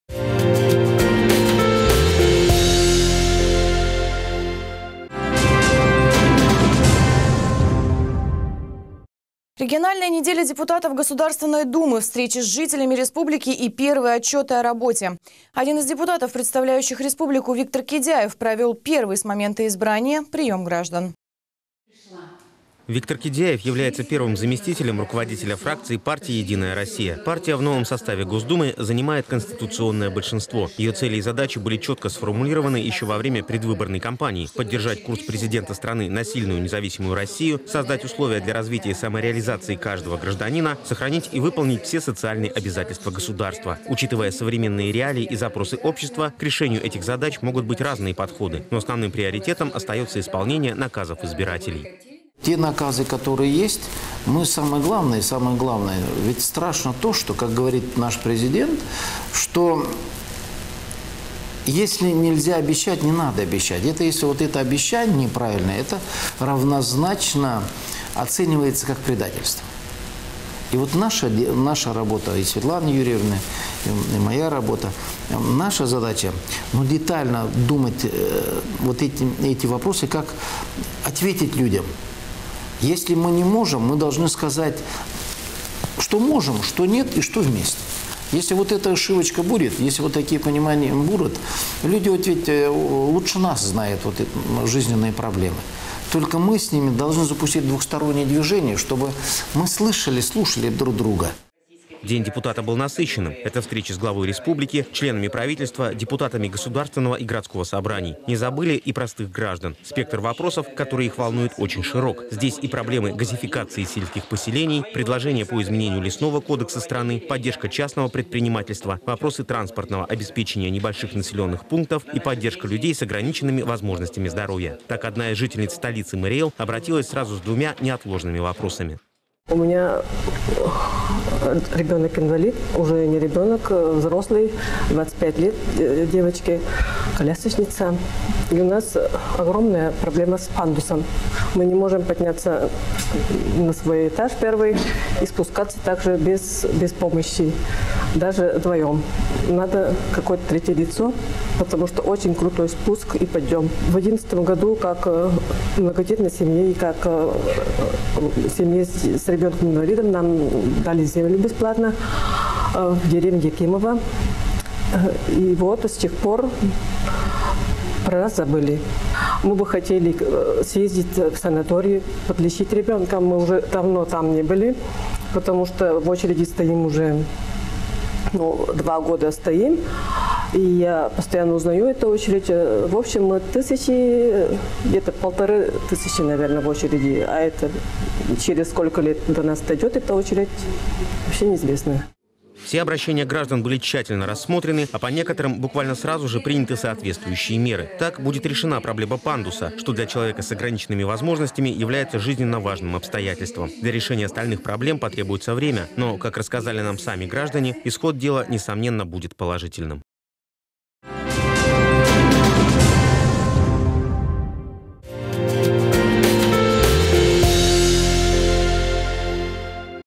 Депутат Кидяев в рамках региональной недели провел прием жителей Йошкар-Олы
Региональная неделя депутатов Государственной думы - встречи с жителями Марий Эл и первые отчеты о работе!

Один из депутатов, представляющих республику - Виктор Кидяев провел первый с момента избрания прием граждан.